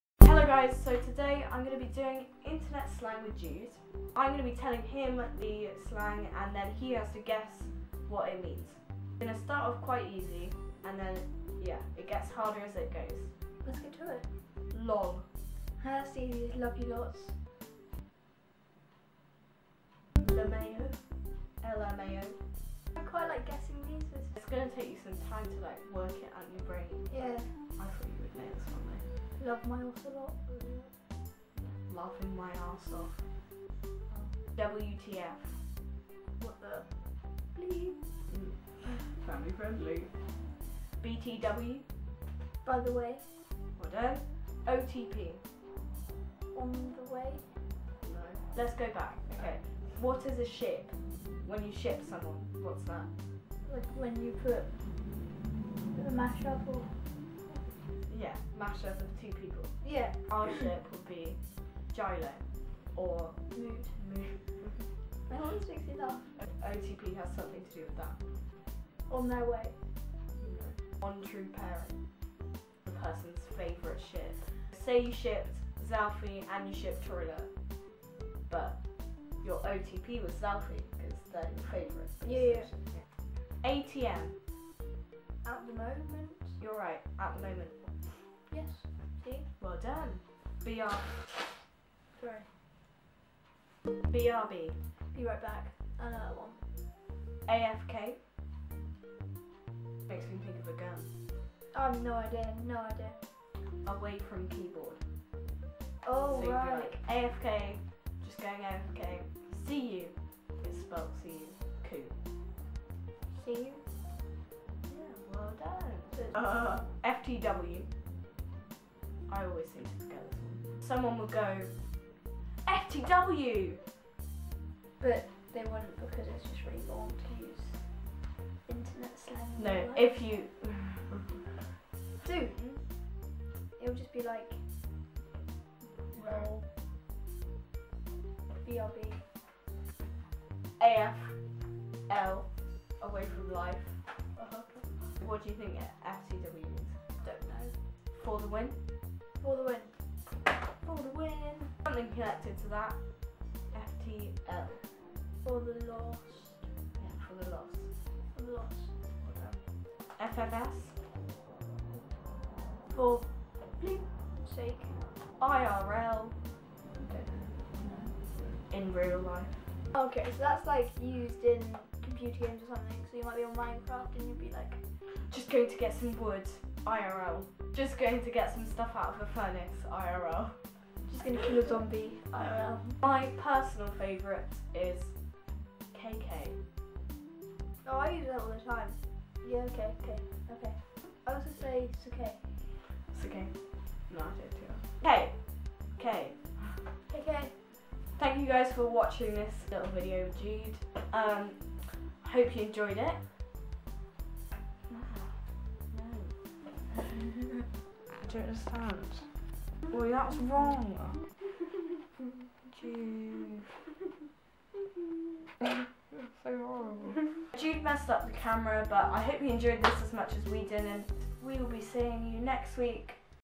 Internet Slang Challenge
Jude tries to guess what internet slang means!
We out!